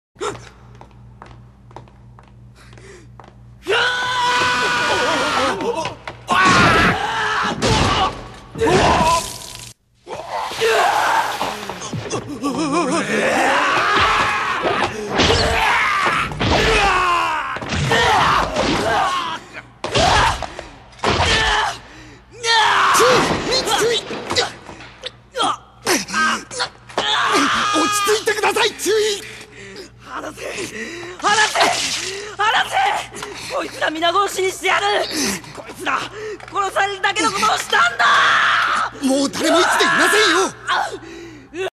Julian goes Berserk
Julian is kinda angry è_é/
from episode 83
thanks to Central-Anime, as always!